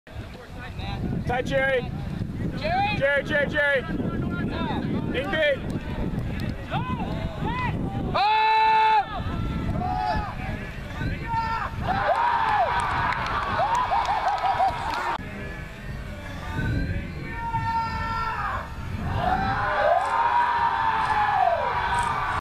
Haymaker Layout - DWTC 2015
Haymaker's Steven Cummings gets horizontal against us. Game recognize game.